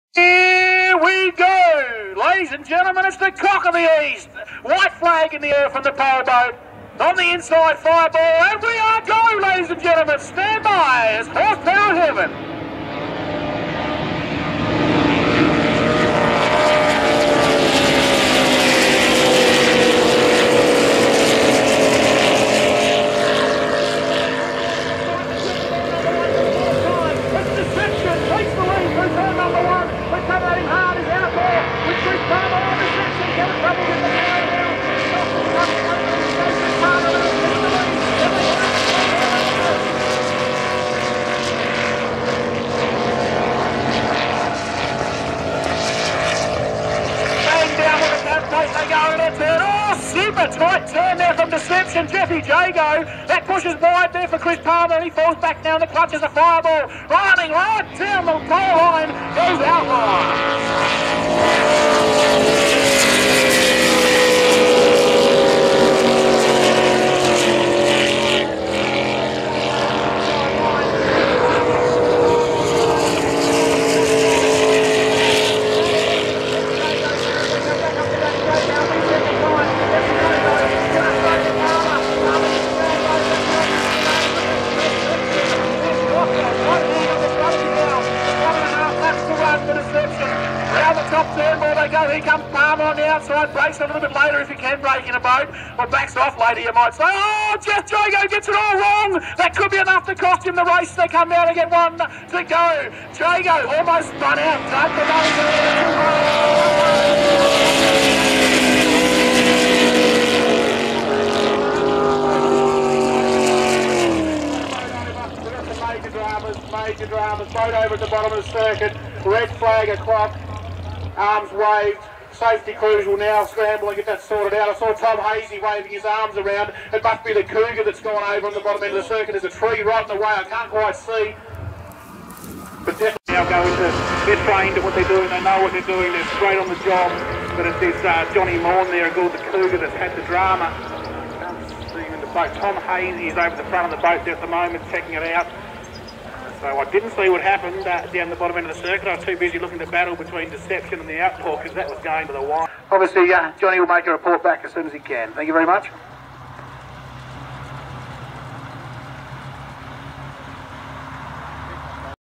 Powerboats, Granton , Tasmania, Cock of the East , 2014
Powerboat action from southern tasmania , January 2014. 6 litre and Unlimited unblown competing. Including the rescue of John Maughan after he was throw out of "Cougar".